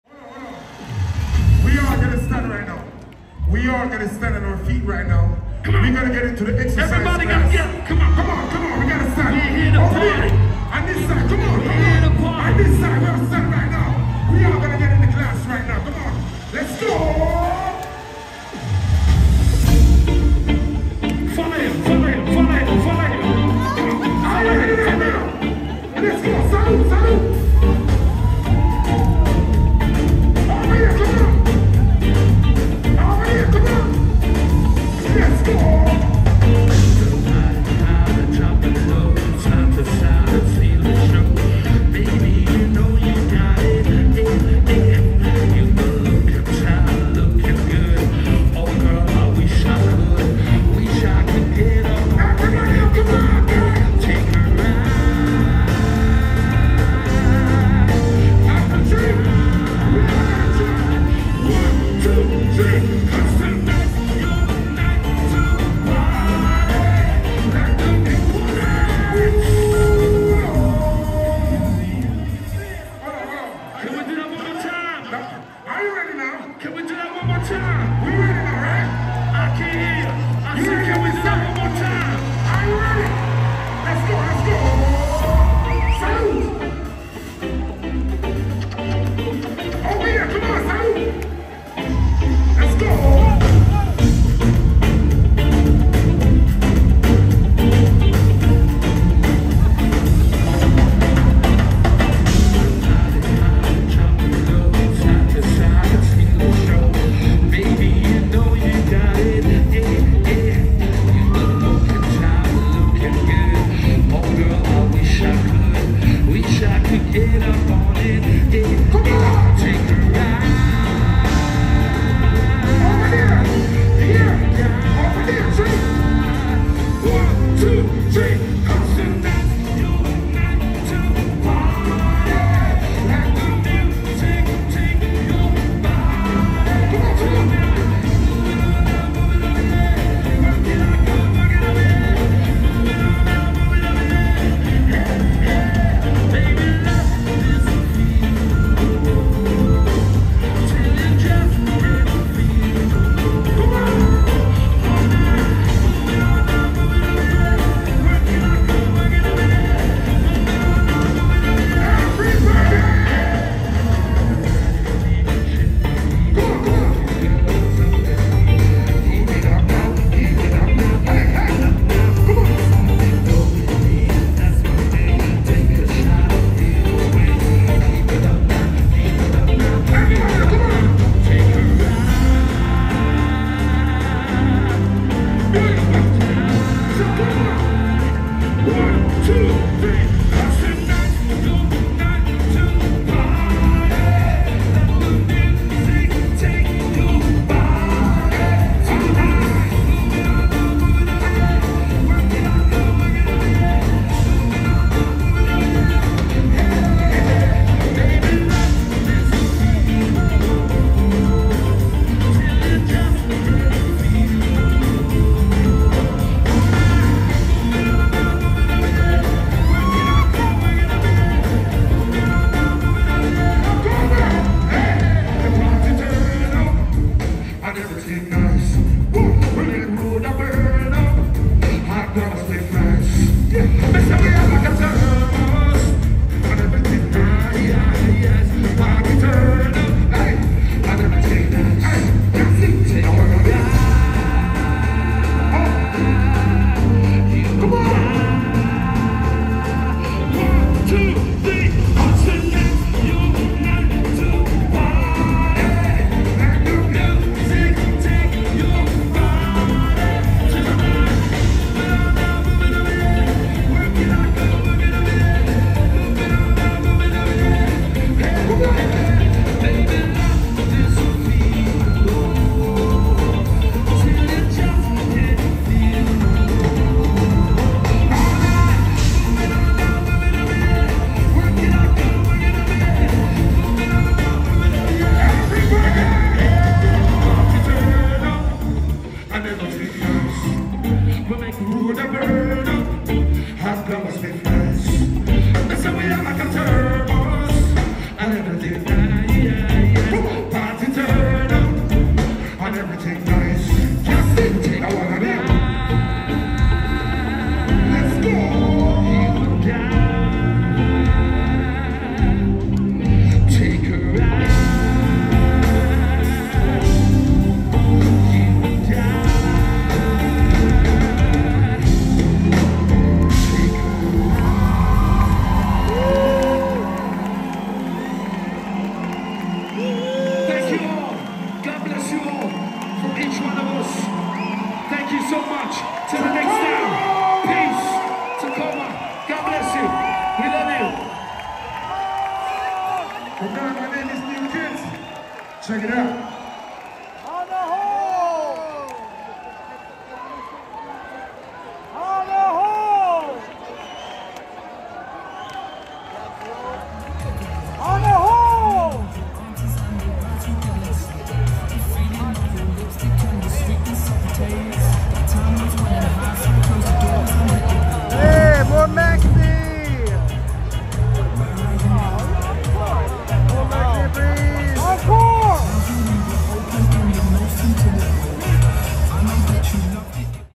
Unknown Song - Maxi Priest Live at The Emerald Queen Casino in Tacoma 7/21/2024
This is Maxi Priest performing an Unknown Song Live at The Emerald Queen Casino 🎰 in Tacoma, Washington on July 21st, 2024!

If you know the name of the song please comment!